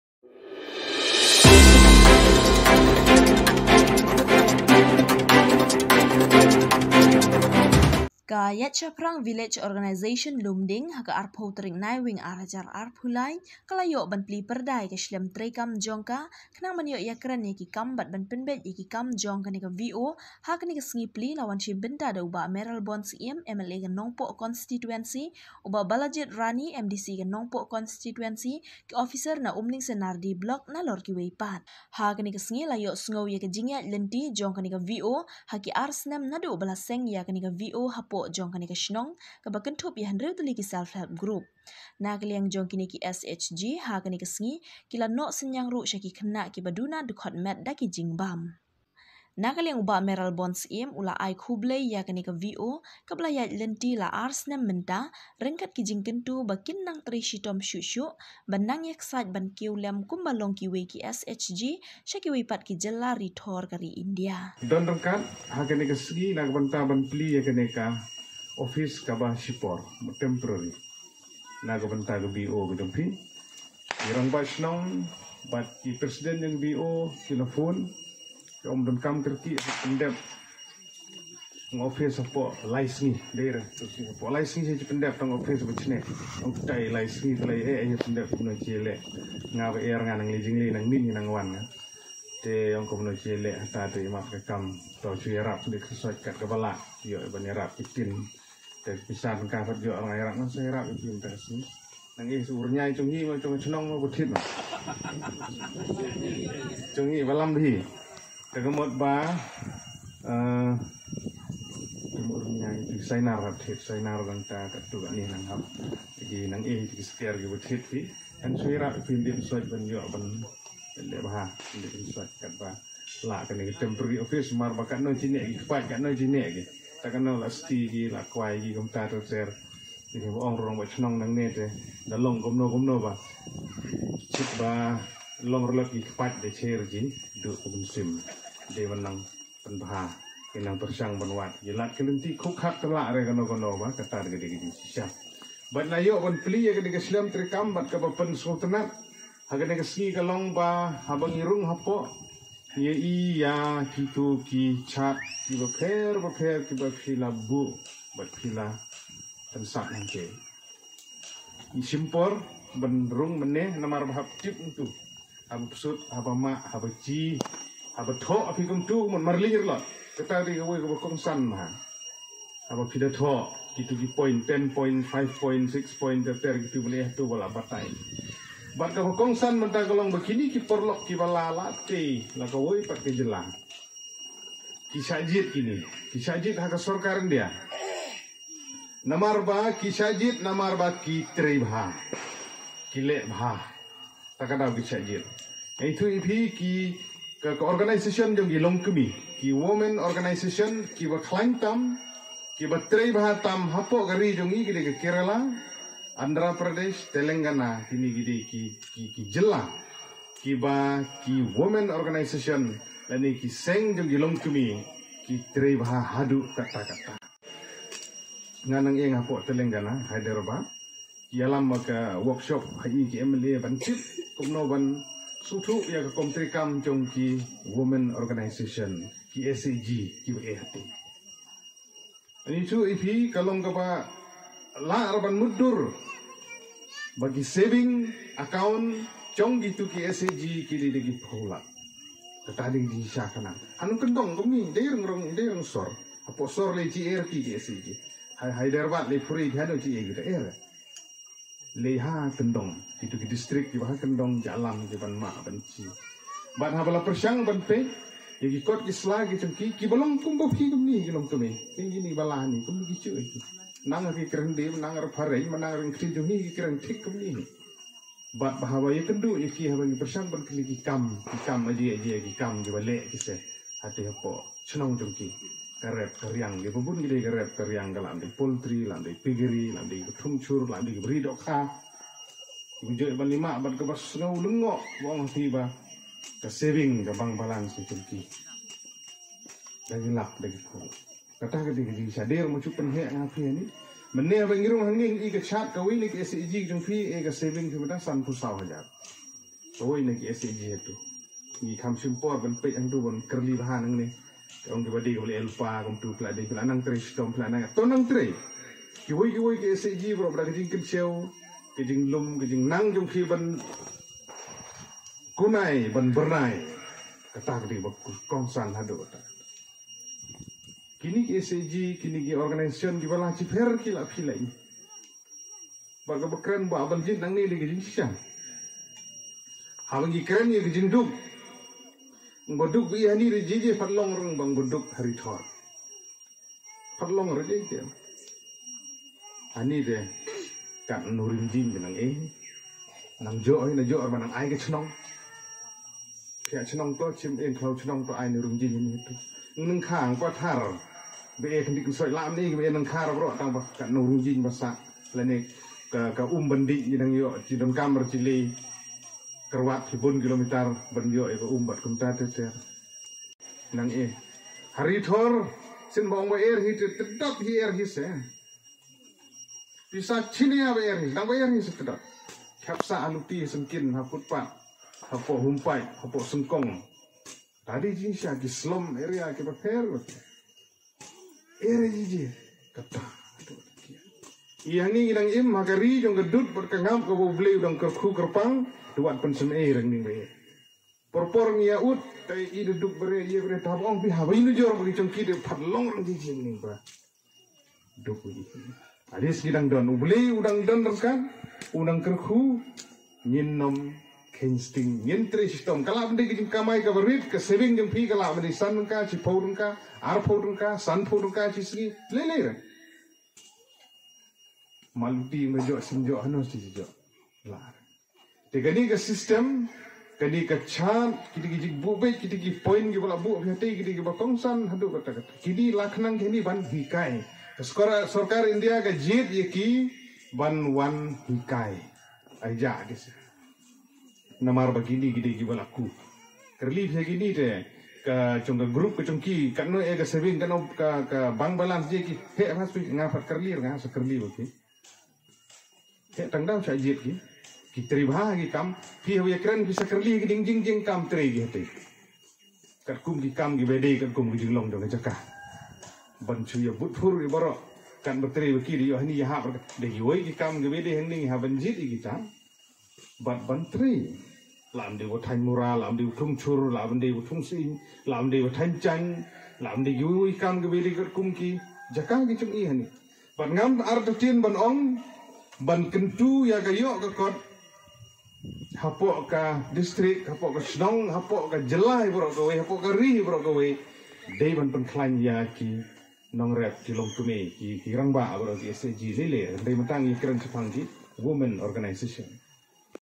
PLIE PYRDA U BAH MAYRAL IA KA SHLEM TREIKAM JONG KA IAID SHAPHRANG VO LUMDIENG
A Local News From Ri Bhoi
KI KHUBOR LAH BHOI LEH DHI I
LAH CHILING CHA CHILING KA THEM RI BHOI BAIEID